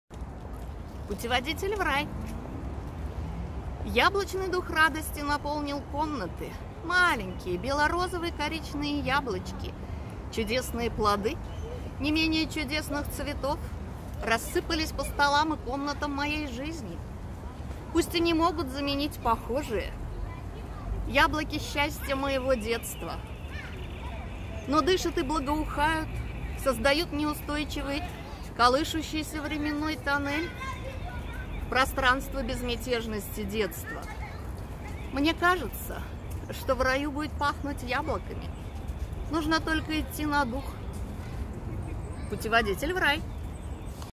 Путеводитель в рай
стихи, верлибр
Яблочный дух радости наполнил комнаты
Маленькие бело-розовые коричные яблочки
Чудесные плоды не менее чудесных весенних цветов
Рассыпались по столам и комнатам моей жизни –
пусть и не могут заменить похожие
Яблоки счастья моего детства,
Но дышат и благоухают –
Создают живой неустойчивый
колышущийся временной тоннель
В пространства безмятежности детства
Мне кажется, что в раю будет пахнуть яблоками
Нужно только идти на дух